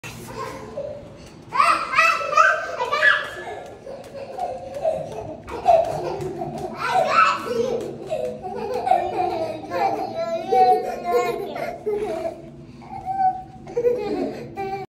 Funny Naomi and David are laughing. Cute siblings relationships!
In this video there were such things and brands:
Doll - Jc.Toys

In this video kids are:
Naomi - 3 years 1 month old
David - 1 year 9 months old